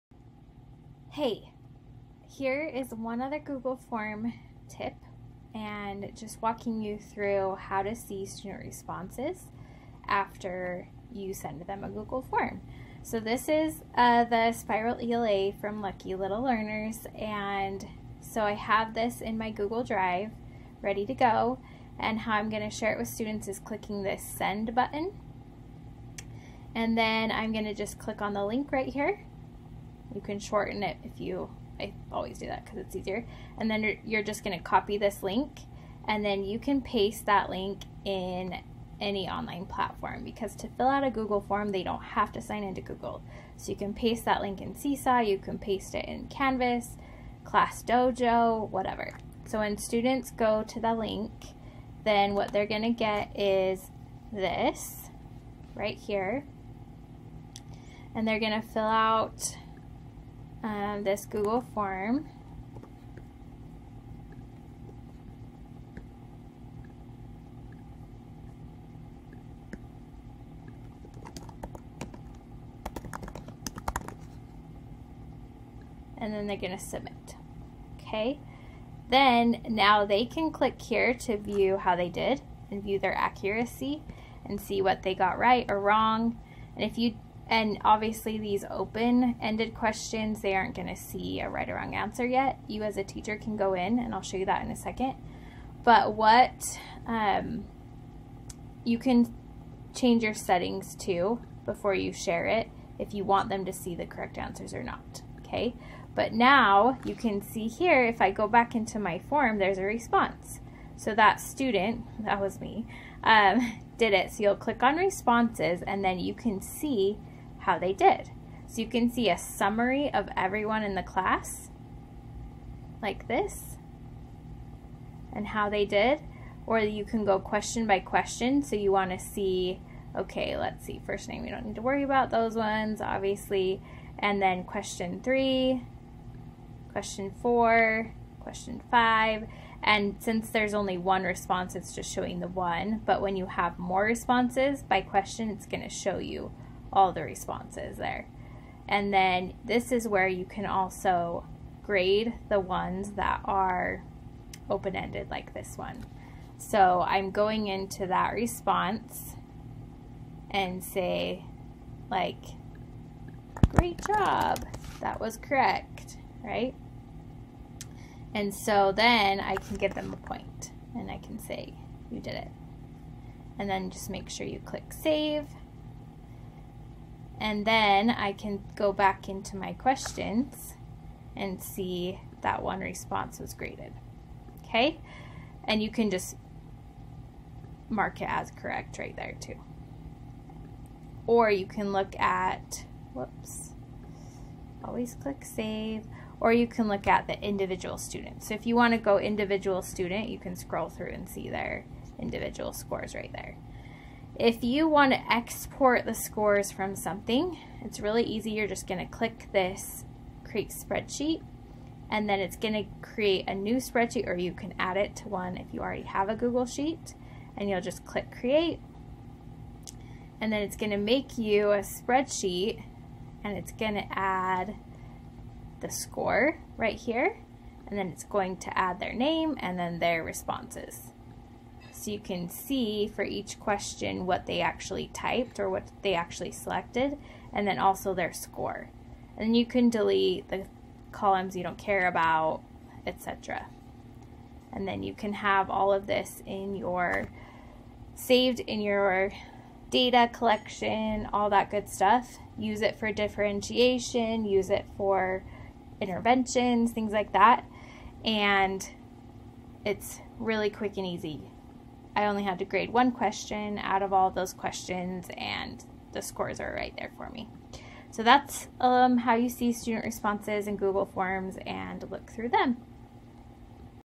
Student Responses in Google Forms
You've assigned a Google Form to your students but now what?  This video goes over how to see your students' responses after they complete a form.